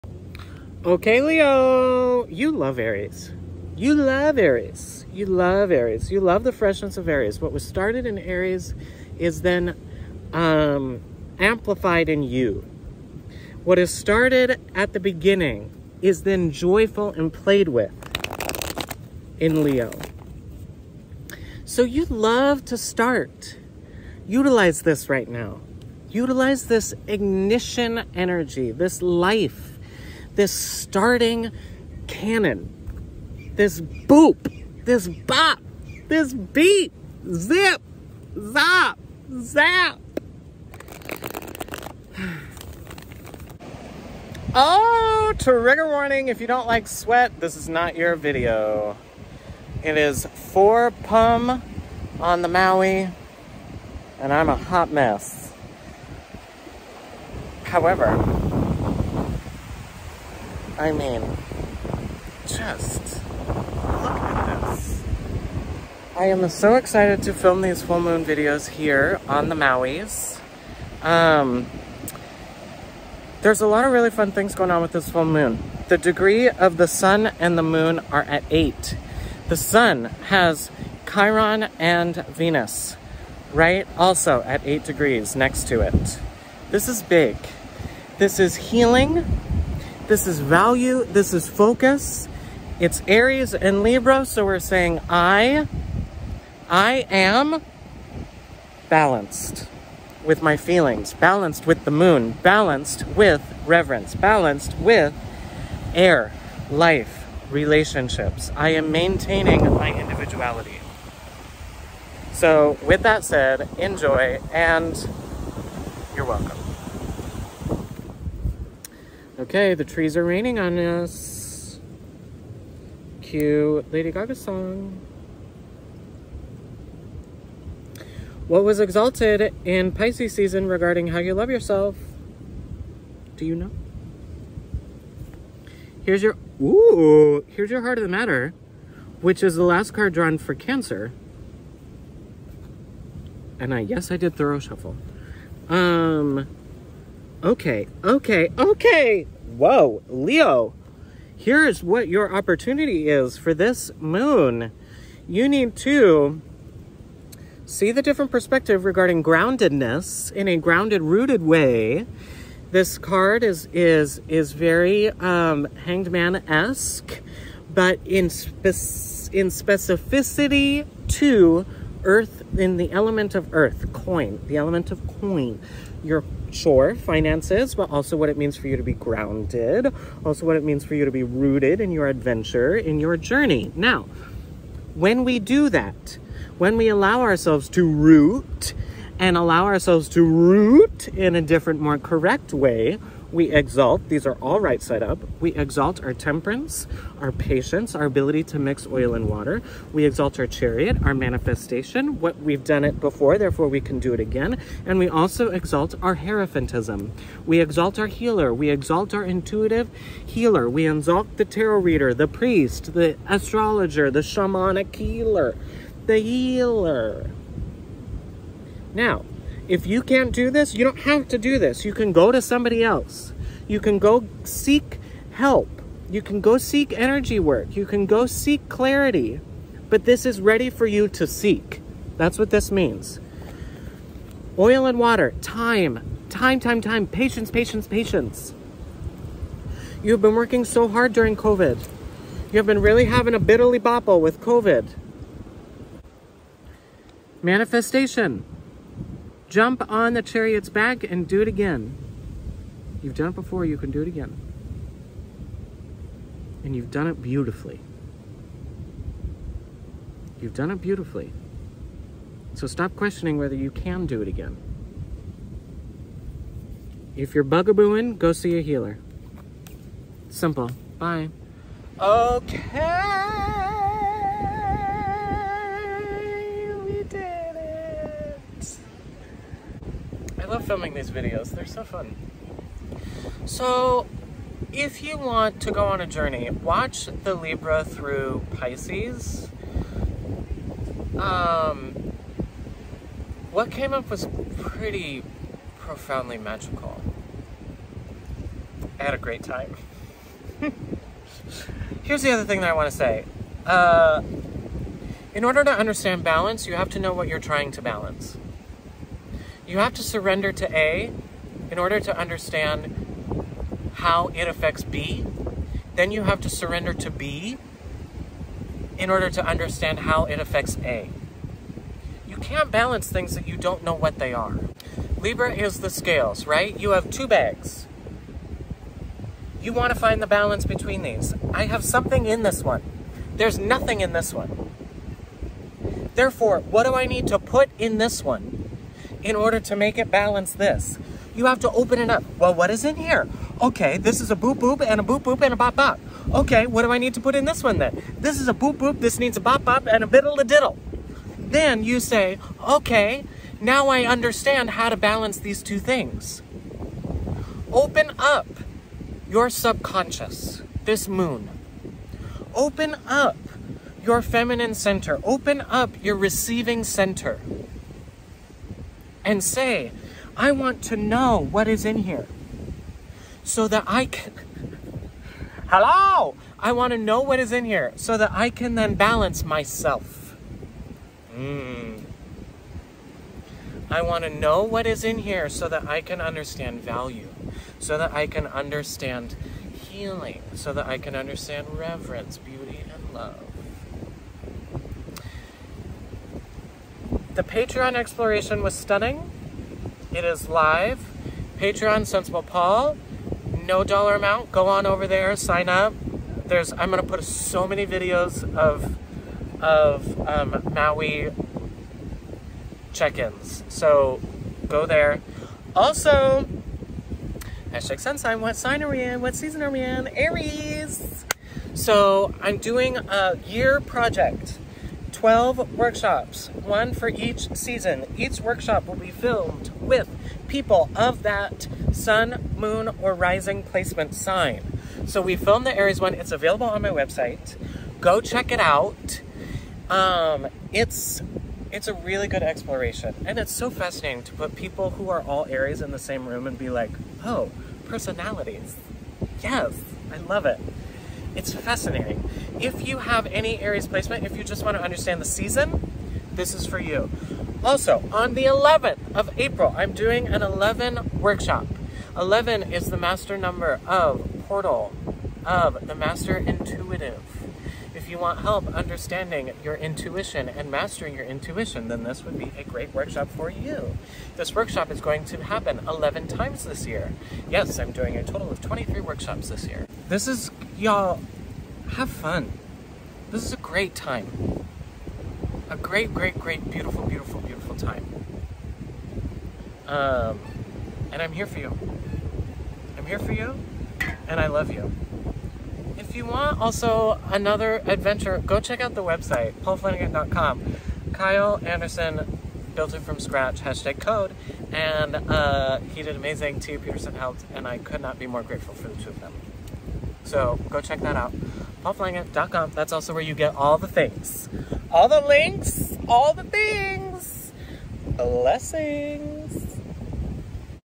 LEO FULL MOON READING
Hello Leo - Here’s your reading for this beautiful, balanced, insightful and uplifting FULL LIBRA MOON!

Watch the videos of your sun, moon and rising sign for your wholistic check in. And if your want to take the robust journey watch the Libra thru Pisces for some more insight on reflective clarity.

You are so loved and I’m here for you if you want to do a reading.

to schedule appointments, workshops, fun photos, videos and more Tom foolery!